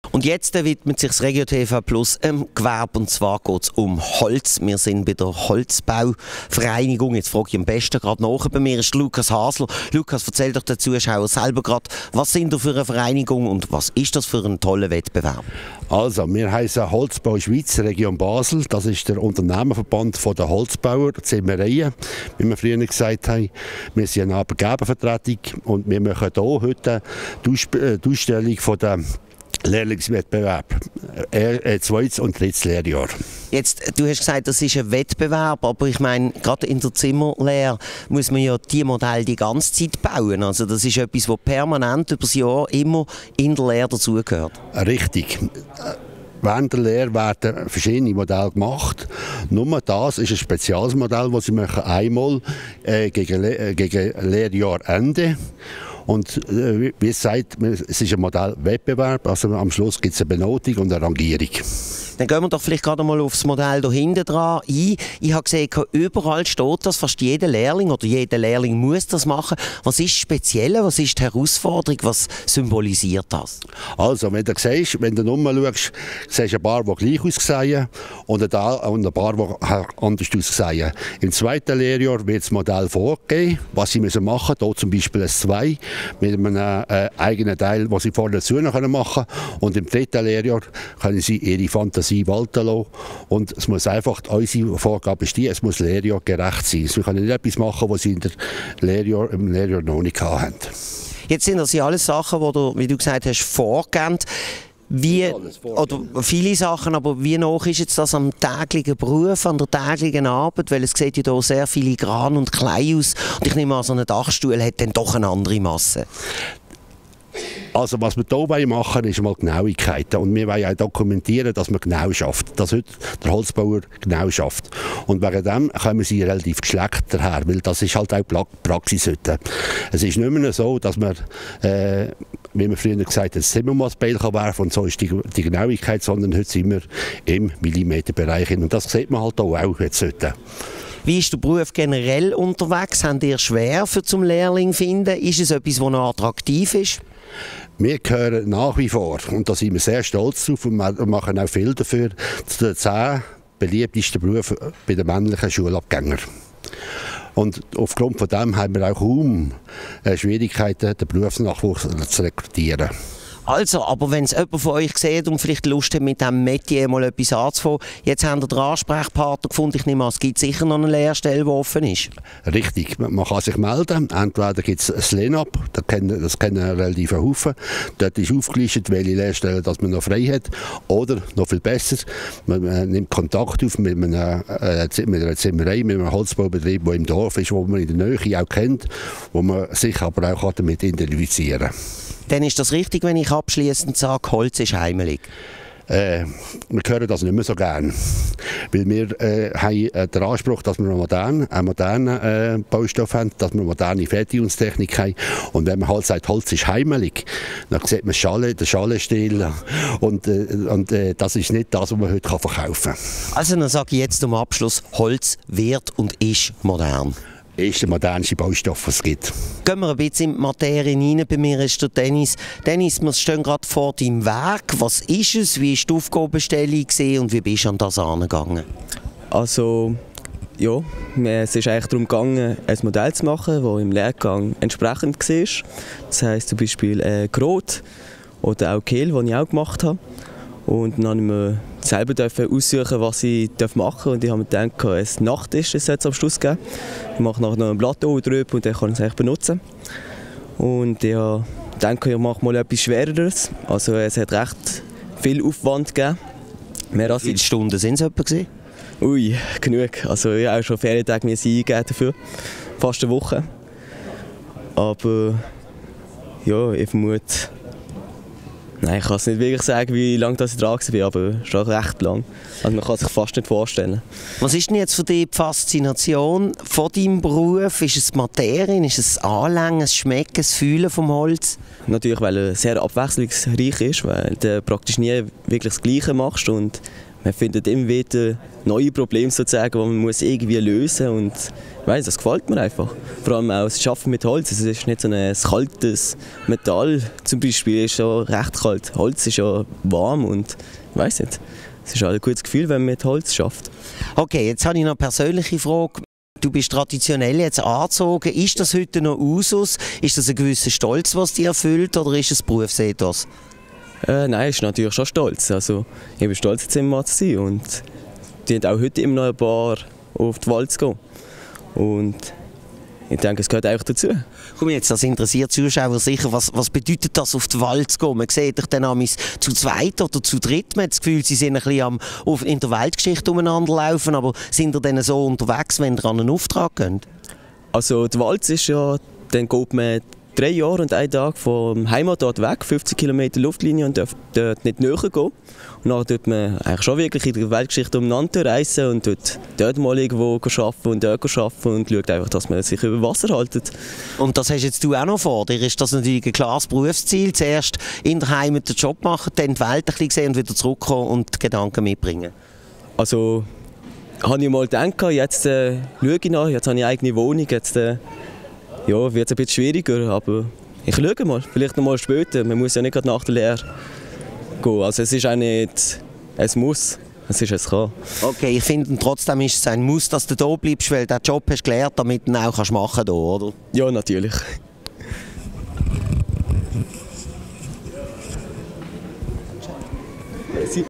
Ausstellung der Zimmerer-Lehrlinge
23. Modellwettbewerb in der MHZ Stutz in Lausen. Engagierte Lehrlinge welche ihren Berufstand im Holzbau Schweiz in der Region Basel leben.